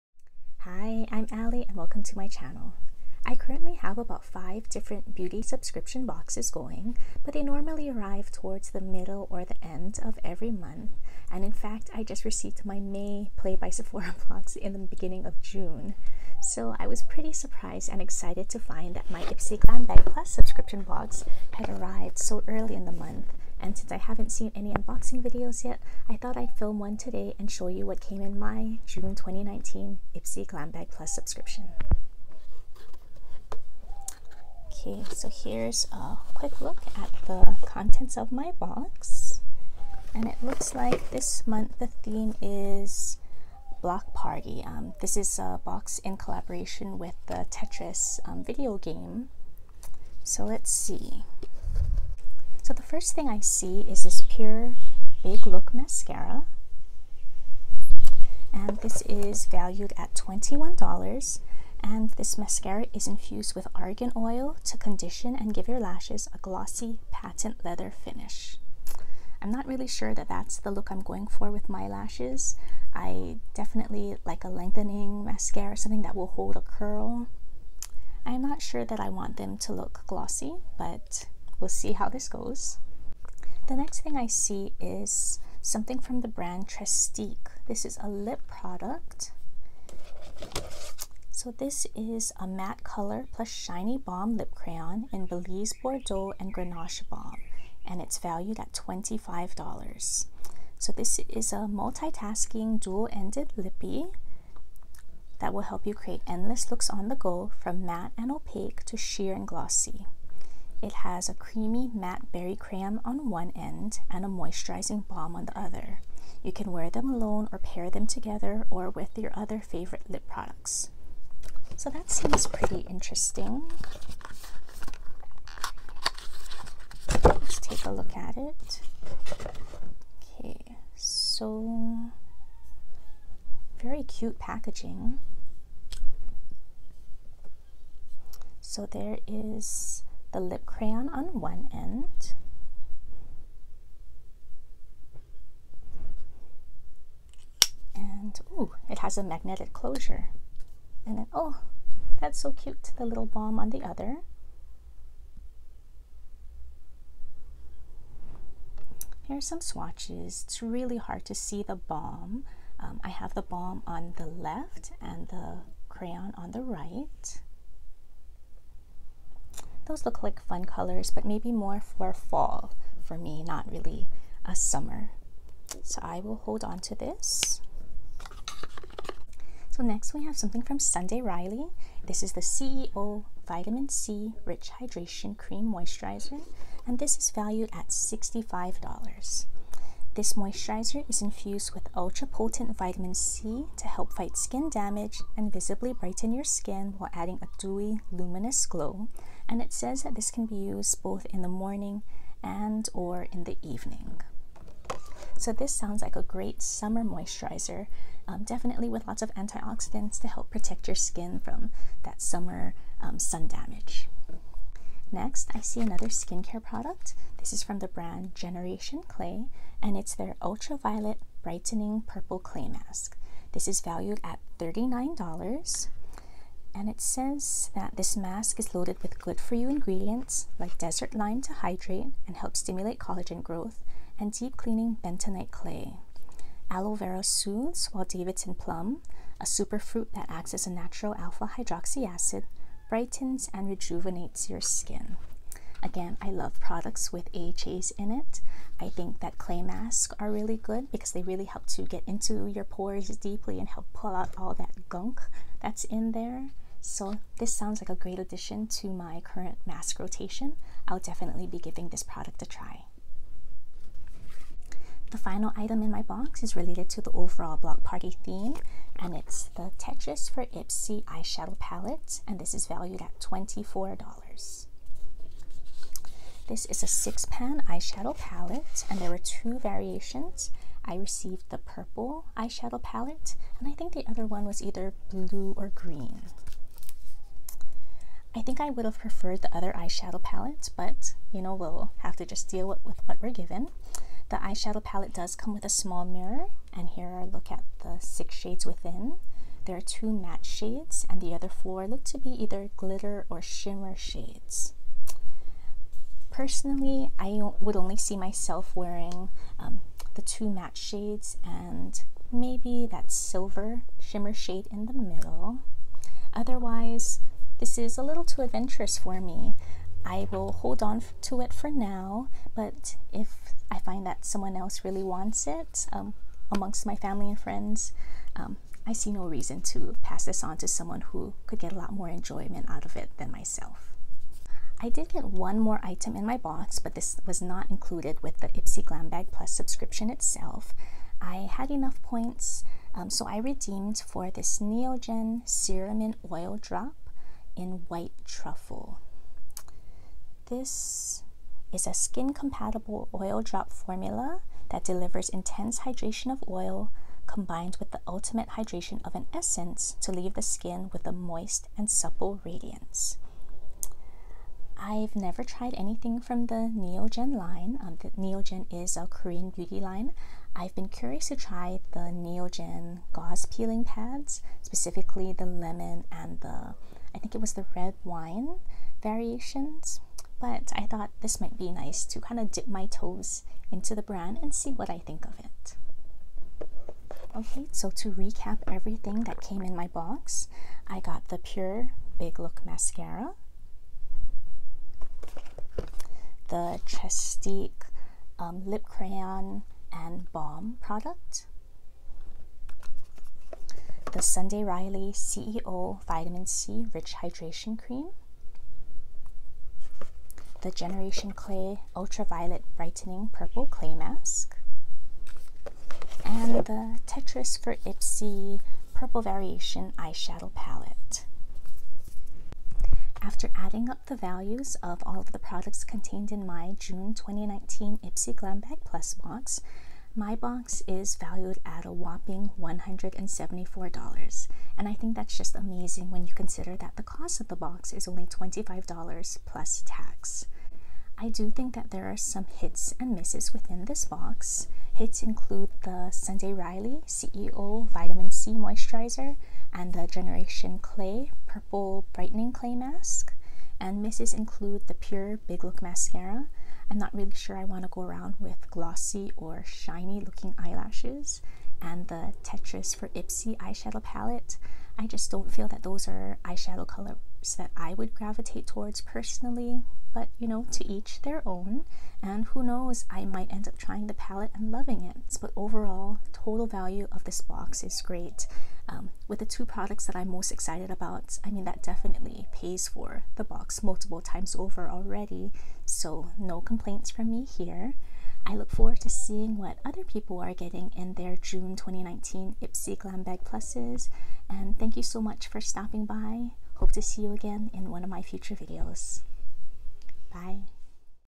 June 2019 Ipsy Glam Bag Plus Unboxing ($174 Value)
I was pleasantly surprised to find a monthly subscription box waiting for me when I arrived home yesterday.  I was even luckier to have the day off of work today so I could film this unboxing video!  I can't wait to see the contents of everyone else's June 2019 Ipsy Glam Bag Pluses...  xoxo Ali

ipsy Glam Bag Plus is a $25/month (+ tax) subscription that offers 5 full-size products based on your personalized beauty preferences.